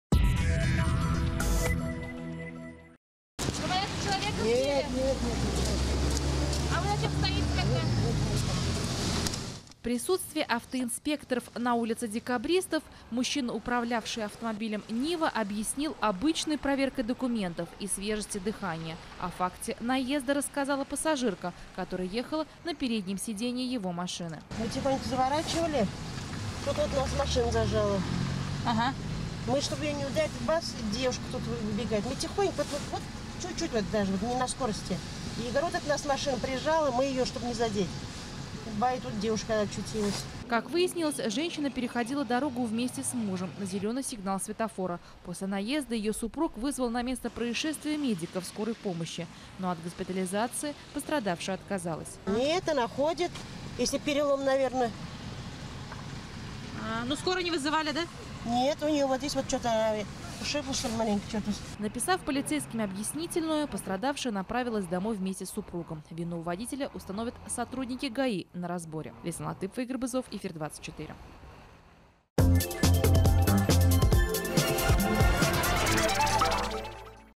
На улице Декабристов сбили женщину на пешеходном переходе
Водитель автомобиля, в общении с журналистами,  факт наезда на пешехода отрицал.